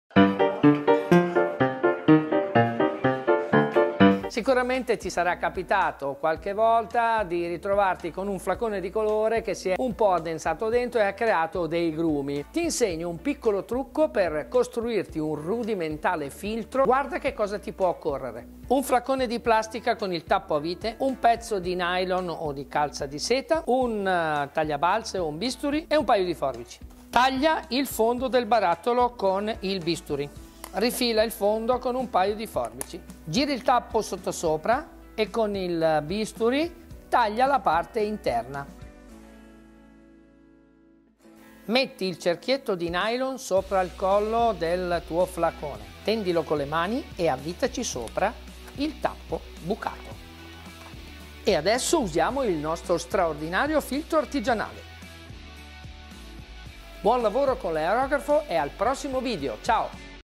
COME realizzare un FILTRO per COLORI ARTIGIANALE
Hai bisogno di filtrare del colore che ha fatto un po' di grumi ma non hai un filtro...

Ho io quello che fa al caso tuo, fermati a guardare questo breve tips&tricks 😊